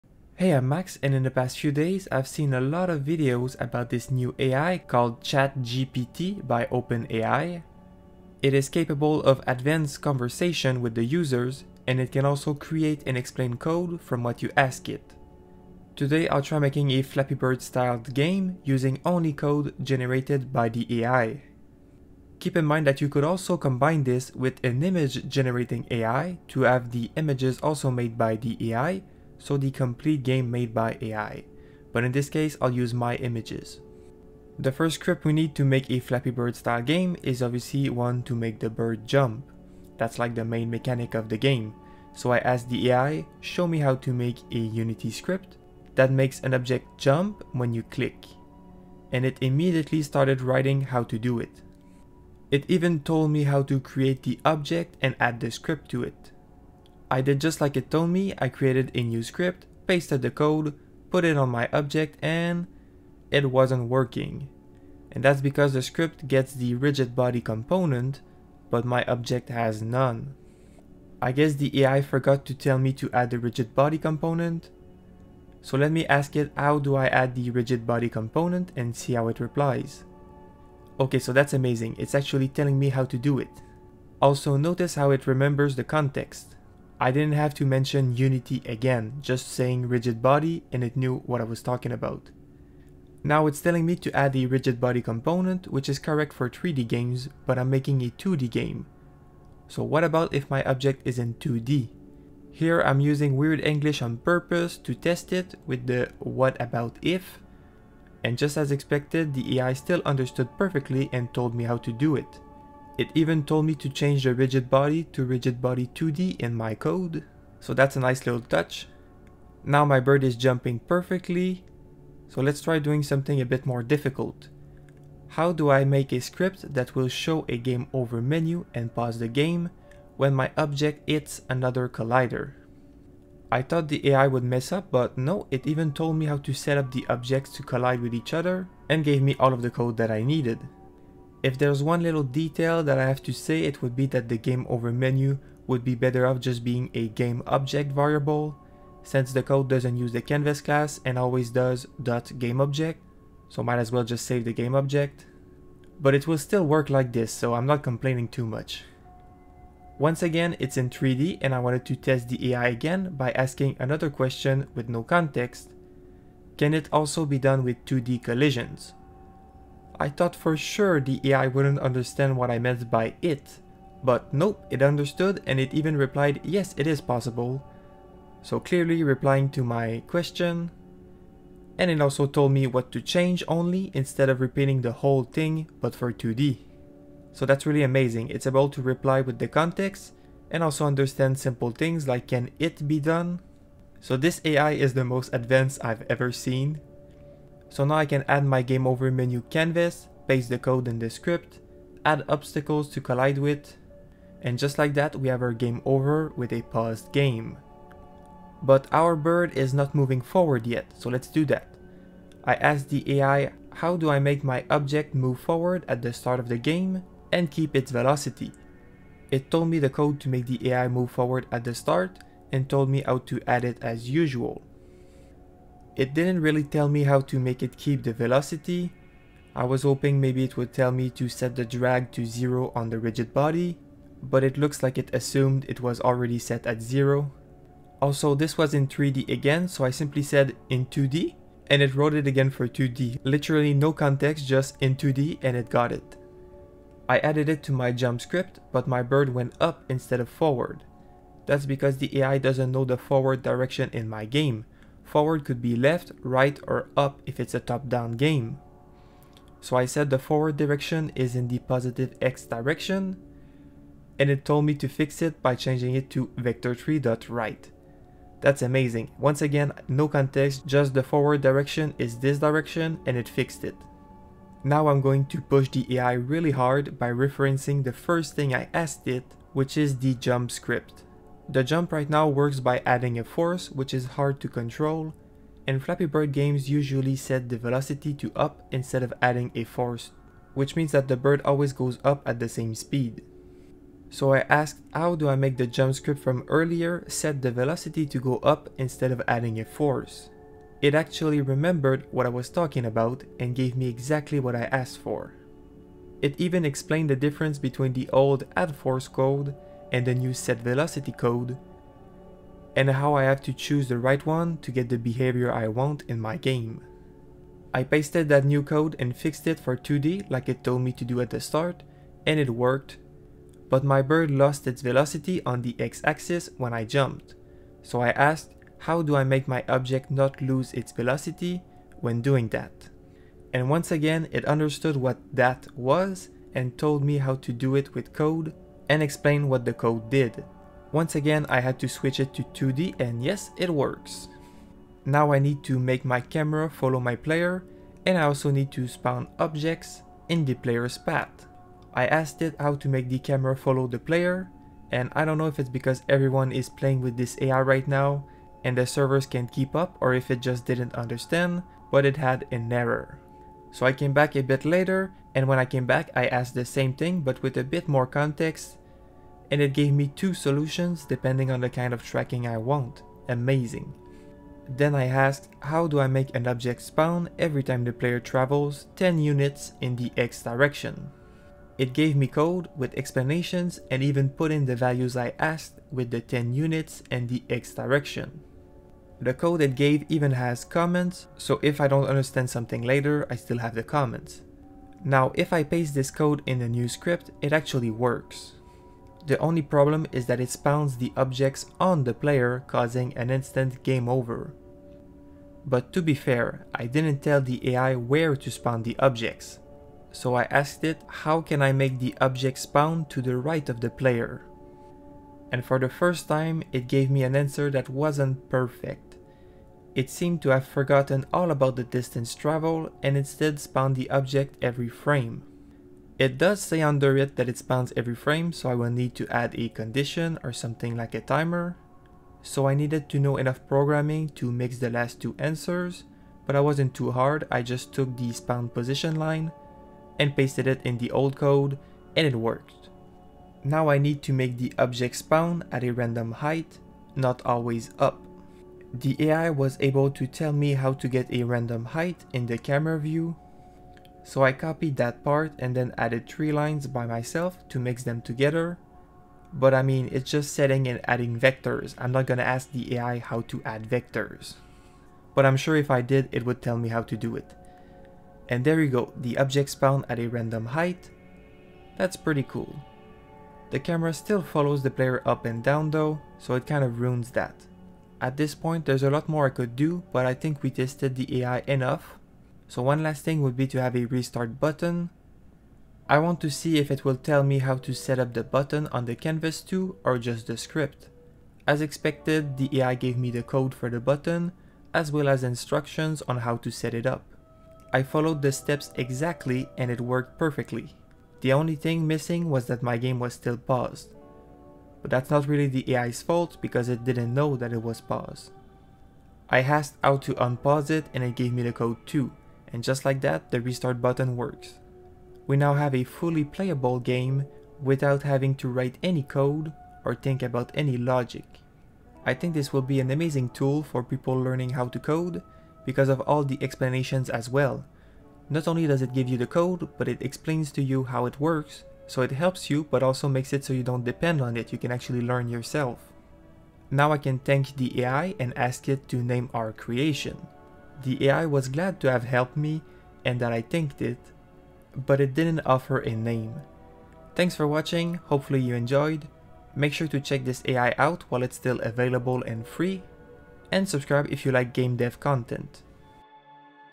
This New AI Can Code Games & Teach You ?!
Chat GPT by OpenAI is a recently released AI that is capable of natural conversation with the speaker. It has knowledge of everything, even how to code most things, so you can ask it to code something for you and it will. I tested it out by making a game using only code written by the AI.

Even better than just writting code, the AI is able to describe in text how it works, what it does, the parameters you might want to change, things to keep in mind / be careful about when using this code, etc. It feels like a teacher ! I think this will be very good for people learning how to code. Rather than google and get code you don't understand and not being able to ask questions, with this AI you will get explanations and if there is something you don't understand, you can ask more about it. You can also ask for modifications and it will tell you how to do them.

AI Link ( Go fast because it is public and free right now but it might not stay like this forever )

0:00 Intro
0:35 Jump Script
2:05 Game Over
3:30 Move Forward
4:45 Set Velocity Jump
5:25 Keep Velocity Jump
5:50 Camera Follow Player
6:20 Spawn Obstacles
6:50 Spawn To The Right
7:25 Random Height In Camera
8:05 Restart Button
8:40 Outro